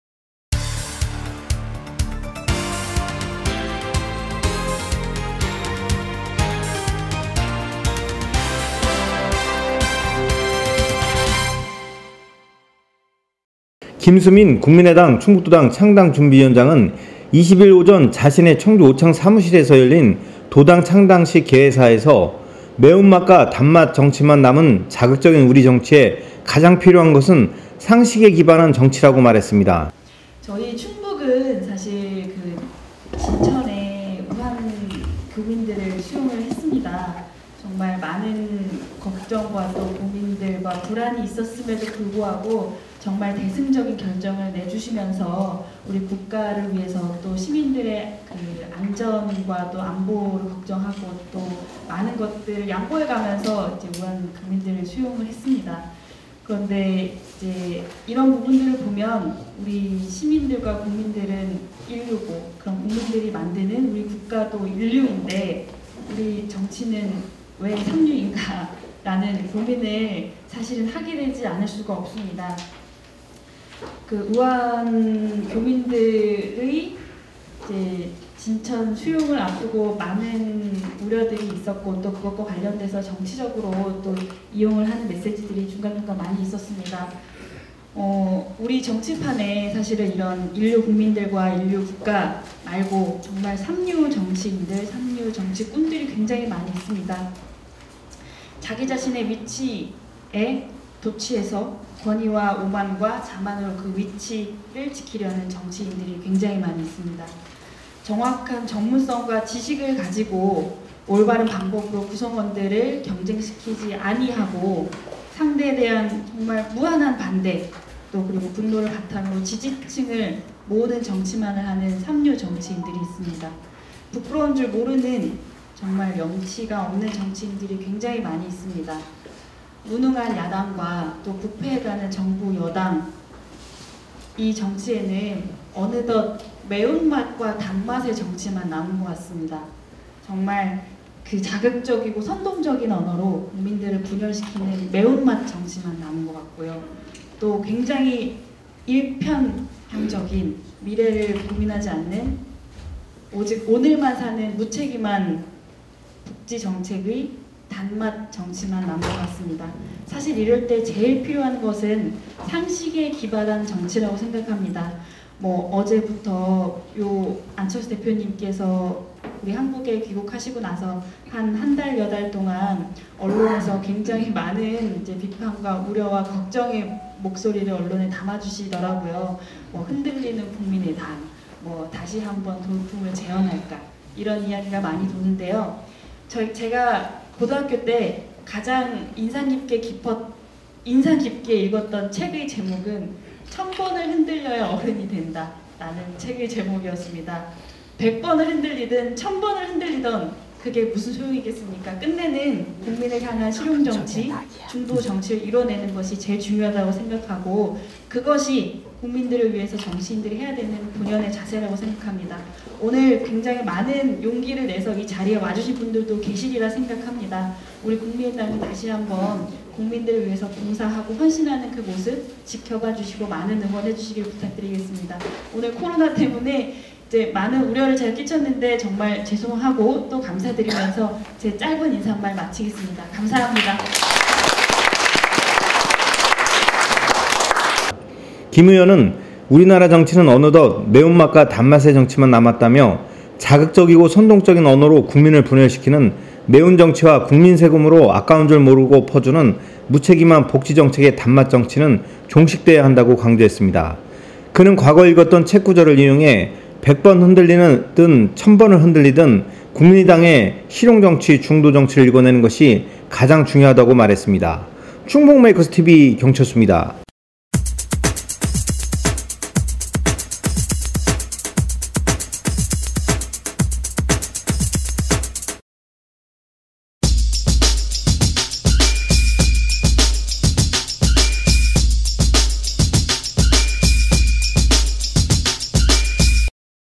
[cbMTV]김수민 국민의당 충북도당 창준위원장 "상식의 정치 필요"
[충북메이커스TV 경철수 기자]김수민(사진영상) 국민의당 충북도당 창당준비위원장은 22일 오전 자신의 청주 오창사무실에서 열린 도당 창당식 개회사에서 '매운맛'과 '단맛' 정치만 남은 자극적인 우리 정치에 가장 필요한 것은 '상식에 기반한 정치'라고 말했다.
김 의원은 "우리나라 정치는 어느덧 매운맛과 단맛의 정치만 남았다"며 "자극적이고 선동적인 언어로 국민을 분열시키는 매운정치와 국민세금으로 아까운 줄 모르고 퍼주는 무책임한 복지정책의 단맛 정치는 종식돼야 한다"고 강조했다.
그는 과거 읽었던 책 구절을 인용해 "100번 흔들리든 1000번을 흔들리든 국민의당의 실용정치, 중도정치를 일궈내는 것이 가장 중요하다"고 말했다.[충북메이커스TV 경철수입니다.]